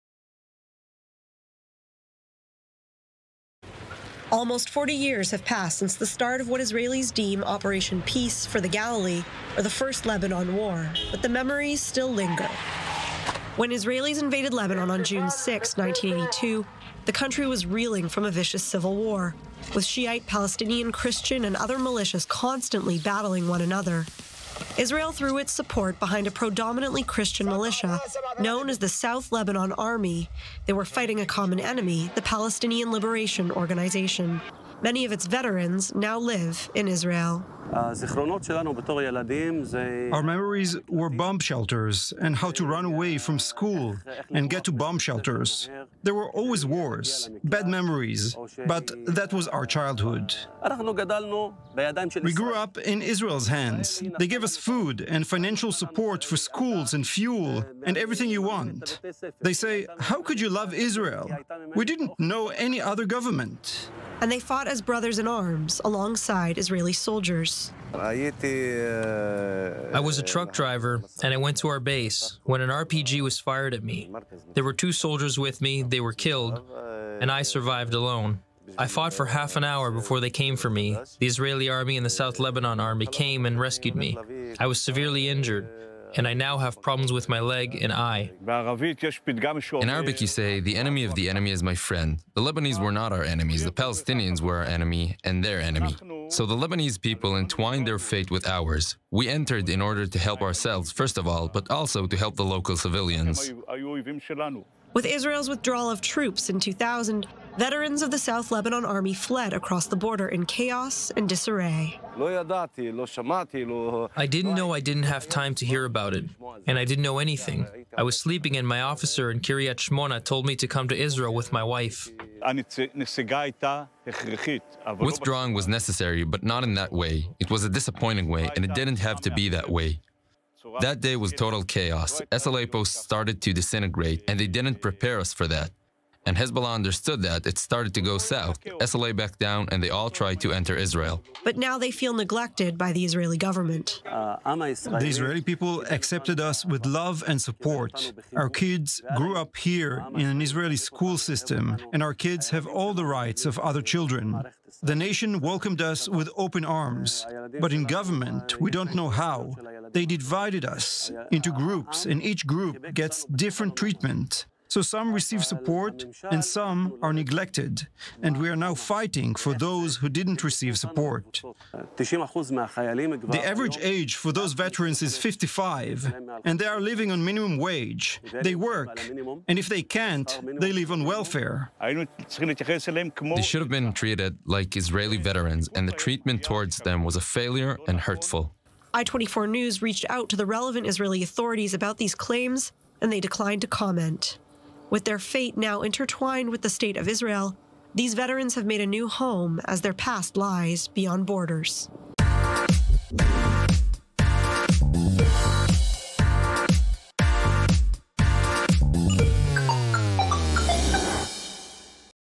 Meet Israel's Lebanese Soliders
STRICTLY SECURITY | It has been 19 years since Israel withdrew from south Lebanon, ending a occupation which was marked by daily conflict. When Israeli troops crossed back into Israel they were accompanied by Lebanese Christians who had served in the Lebanese army. Our Emily Rose has the story.